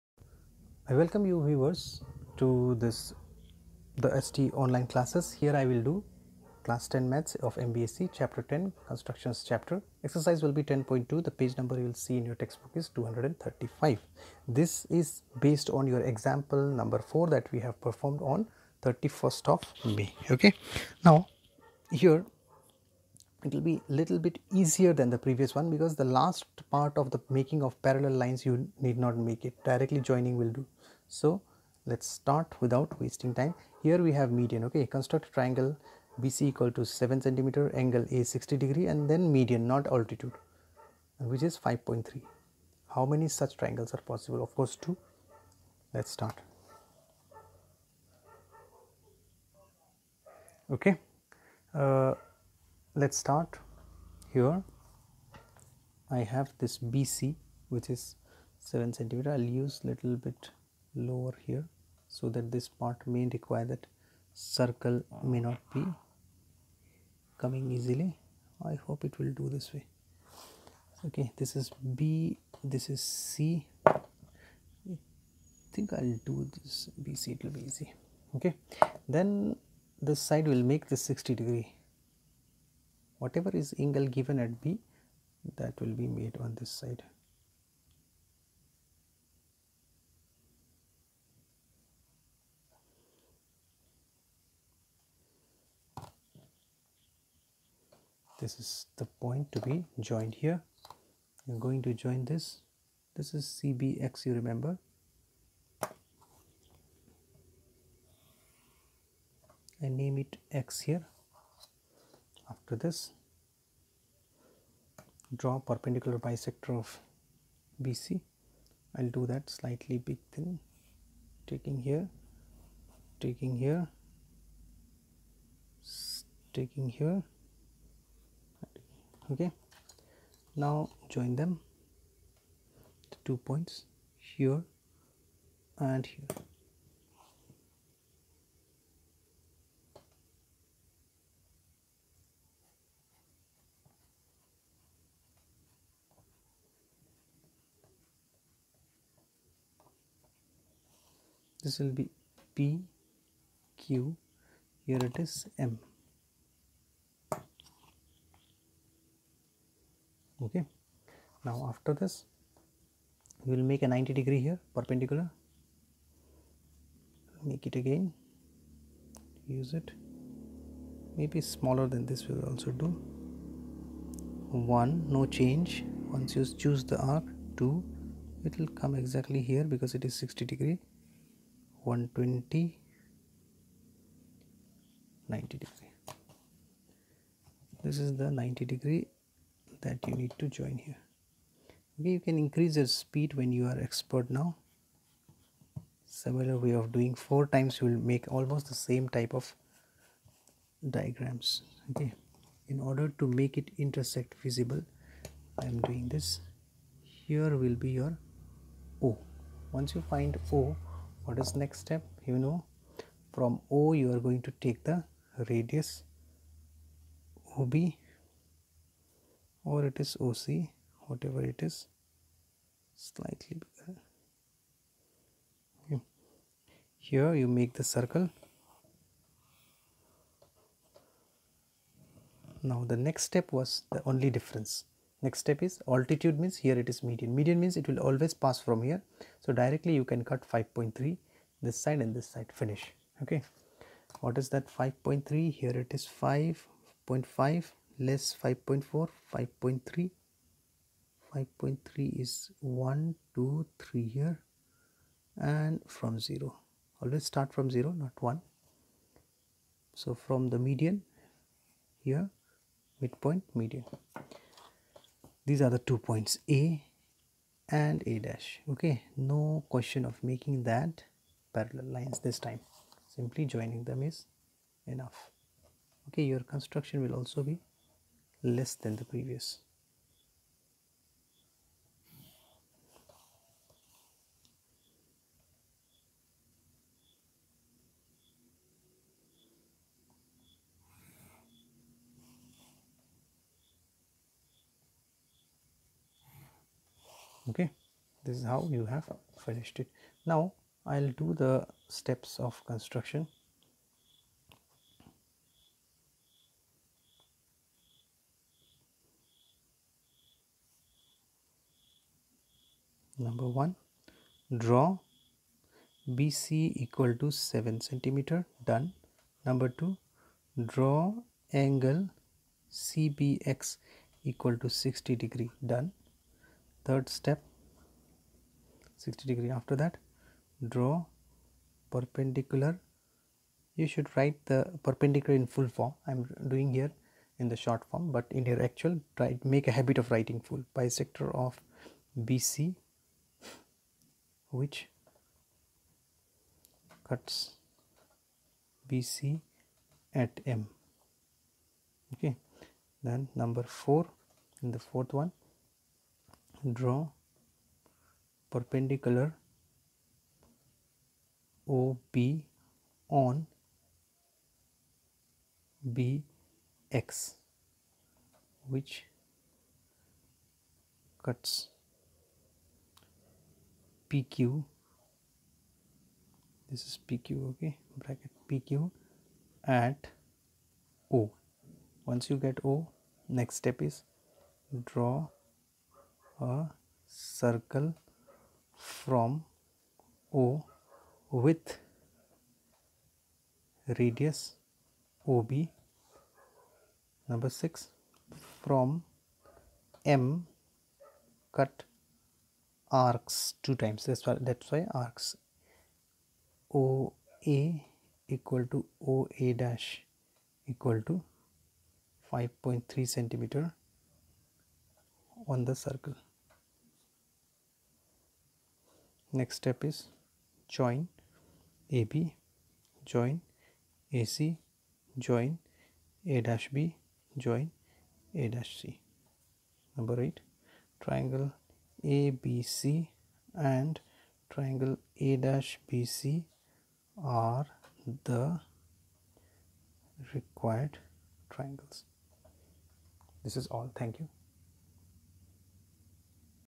Ex 10.2-Q 10: Construct a triangle ABC in which BC = 7 cm, angle A = 60 degree and the median...
Ex 10.2-Q 10: Construct a triangle ABC in which BC = 7 cm, angle A = 60 degree and the median from A on BC is 5.3 cm long. How many such triangles are possible?